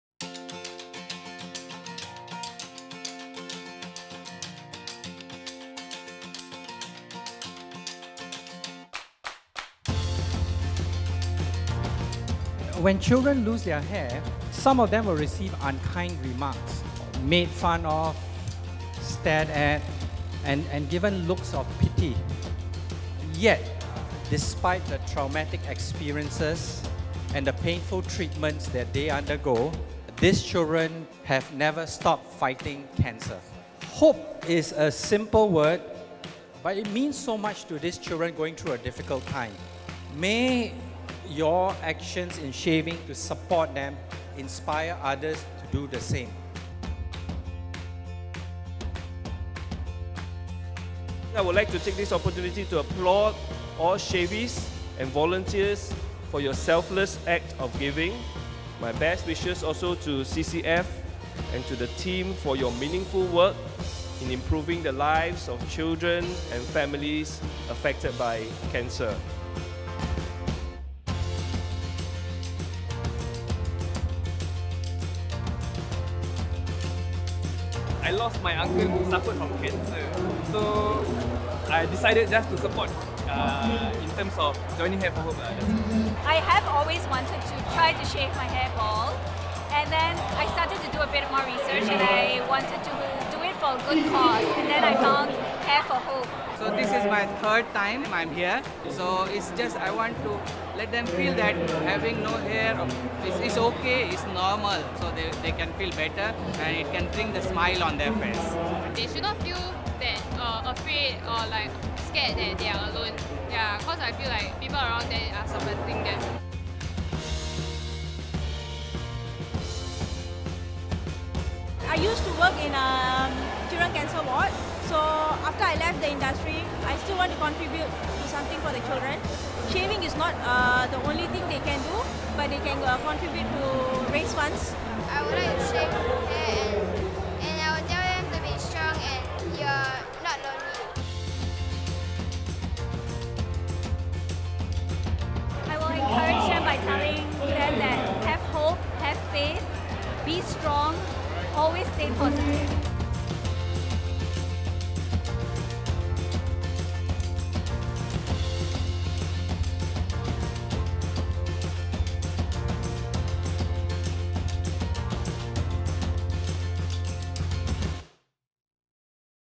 Hair for Hope 2019 - Highlights
CCF held our annual public outreach event, Hair for Hope from May to September this year to raise awareness of childhood cancer in Singapore, and to tell children with cancer that it is OK to be bald.

More than 5,456 shavees volunteered to make a BALD statement as part of the campaign and more than $4.5 million in donations was raised for CCF! What an amazing feat and we're truly humbled by your support.

Hair for Hope may have concluded this year but the battle against childhood cancer hasn't! We hope everyone can continue to support children with cancer and their families. If you would like to register your interest for Hair for Hope 2020, drop us a note at so we can inform you when the campaign launches!